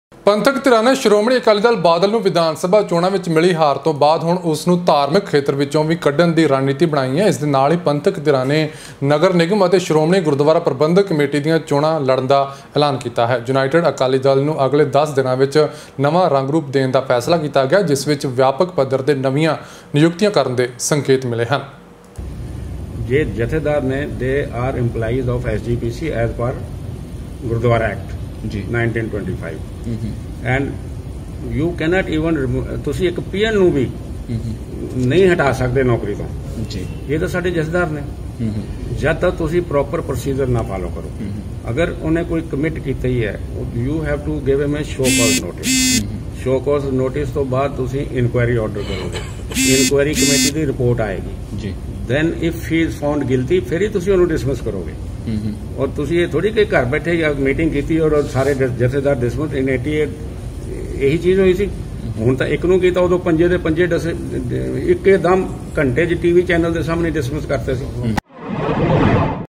Panthic parties plan to contest SGPC and MC elections
ਪੰਥਕ ਧਿਰਾਂ ਨੇ ਬਣਾਈ ਐਸ.ਜੀ.ਪੀ.ਸੀ. ਅਤੇ ਨਗਰ ਨਿਗਮ ਚੋਣਾਂ ਲੜ੍ਹਨ ਦੀ ਰਣਨੀਤੀ